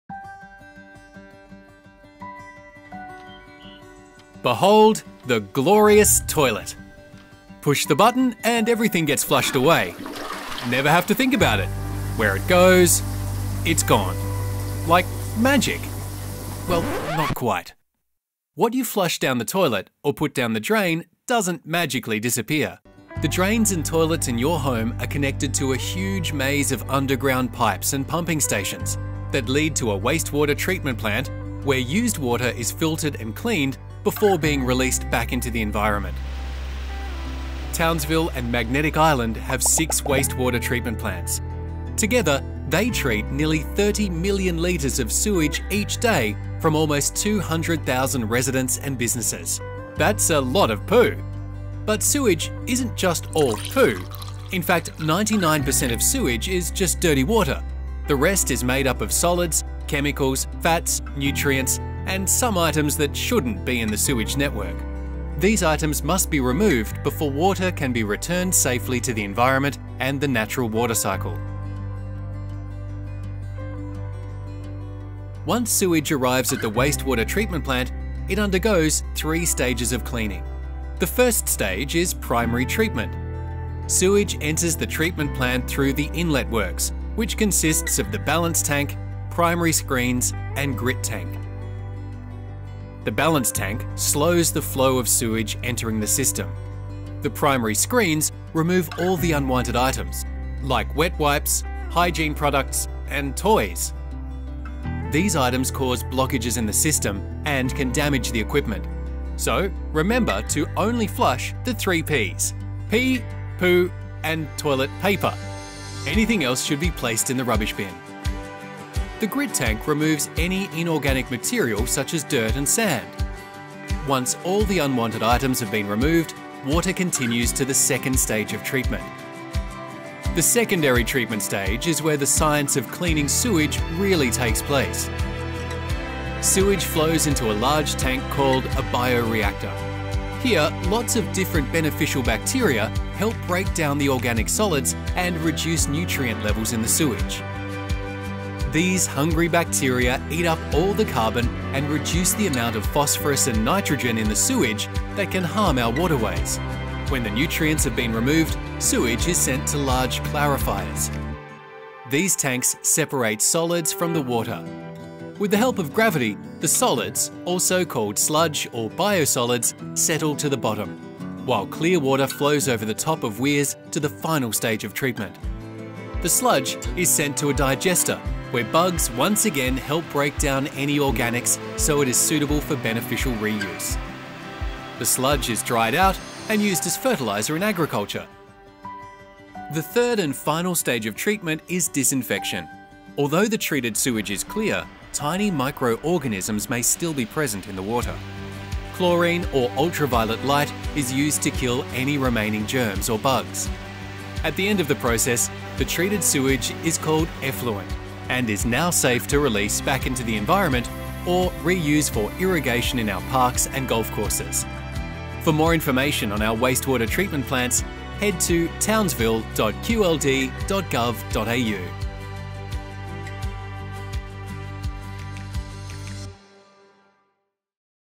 Wastewater Treatment Process
Wastewater is water that has been polluted through human use in the home or commercially. It is transported through the sewerage network to a wastewater treatment plant where it is put through a number of processes so it can be reused.